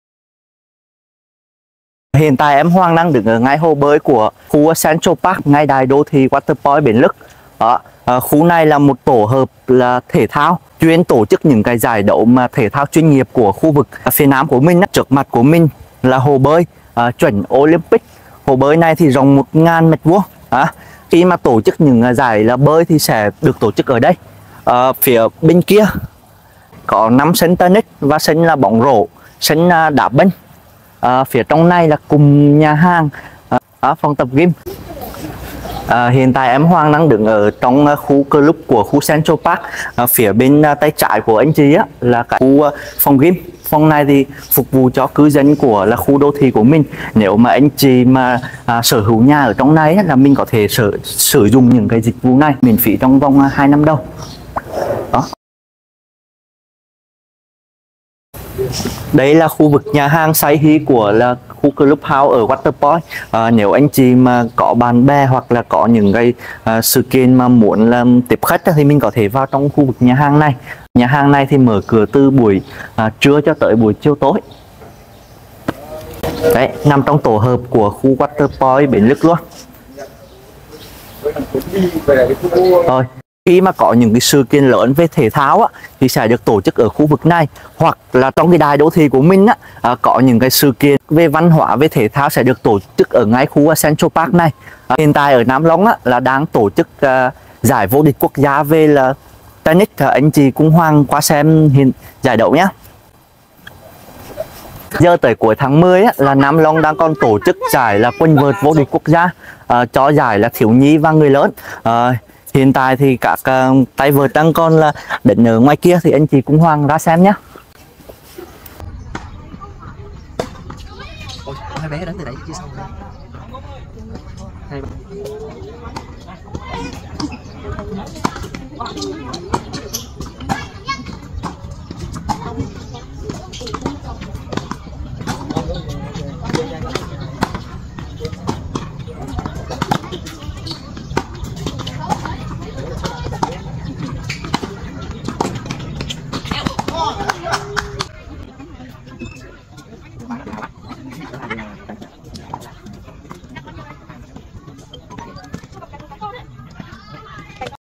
Giải quần vợt vô địch quốc gia 2023 tổ chức tại Waterpoint Bến Lức - Phân khu Central Park
anh chị cùng Hoàng xem khu Central Park có gì nhé.